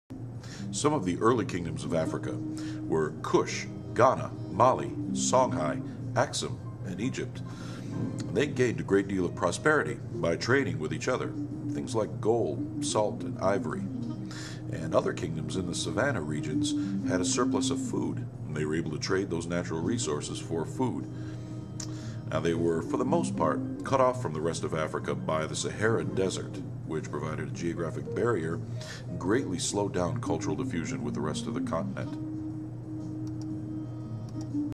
African Trading Kingdoms
Mr. Julian explains the significance of the African trading kingdoms.